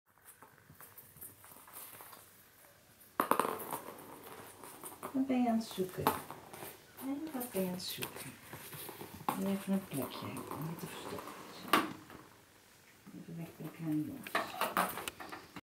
Pet rats playing with a ping pong ball
My Montgomery and Mercurius got a ping pong ball to play with! Do you think they have a shot at a pro soccer career? ;-)